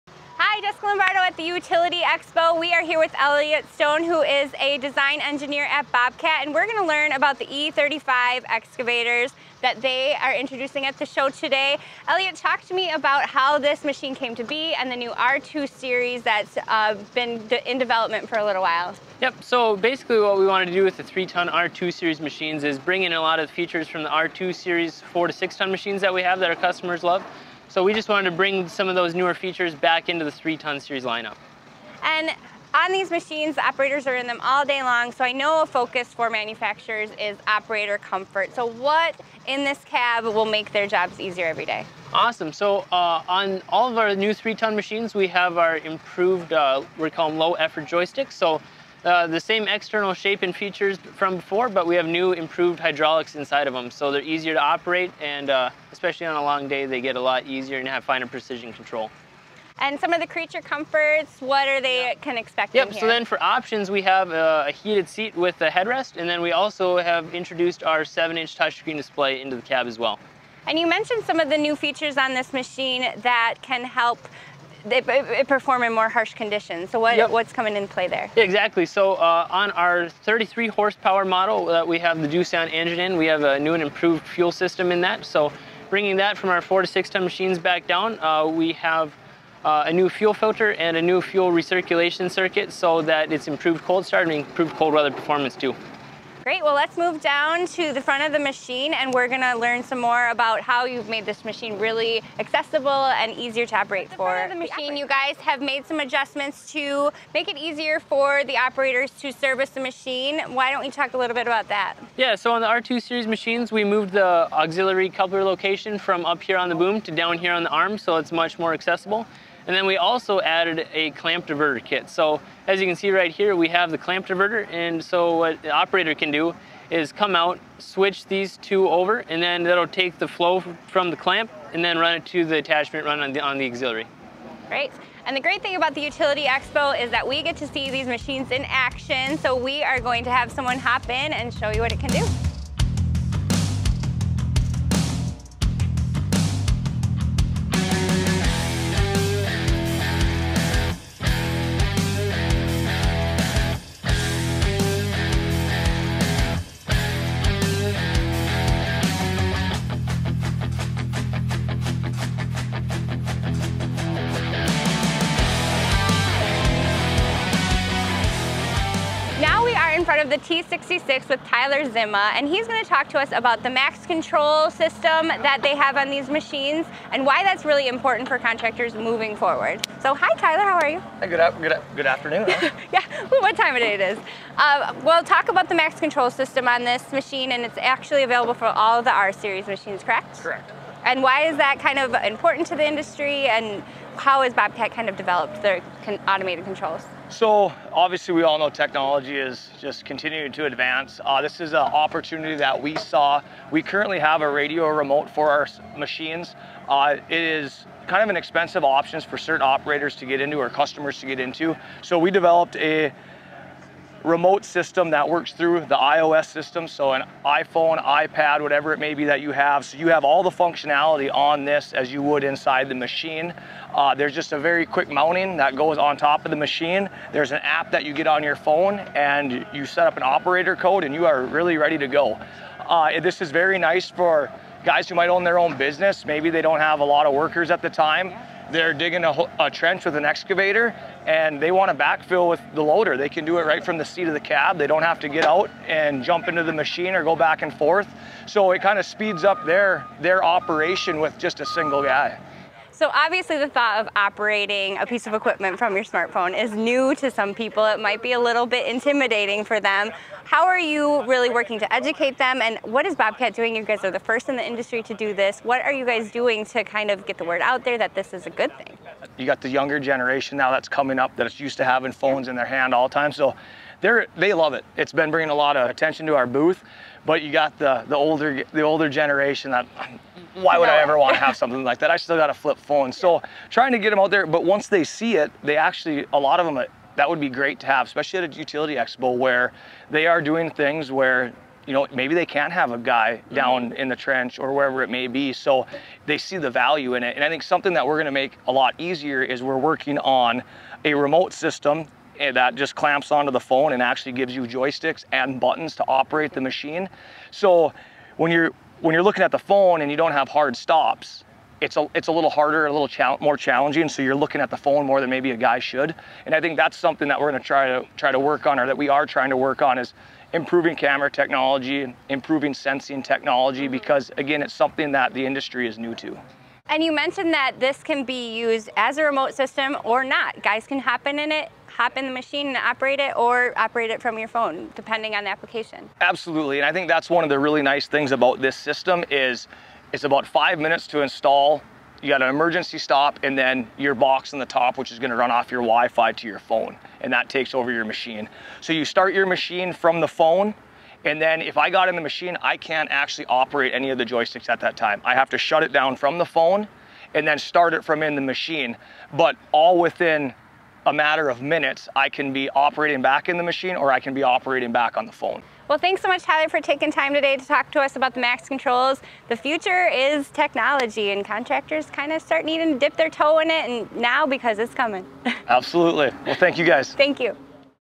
Bobcat shows off its new E35 compact excavator and remote machine control at The Utility Expo 2021
Bobcat engineer, Elliot Stone, walks us through recent improvements made to Bobcat's popular E35 compact excavator. Then, we review Bobcat's Maxcontrol technology, which brings remote control capability to its skid steers and compact track loaders as a factory option—but the coolest part? You can control the machines with your iPhone or iPad.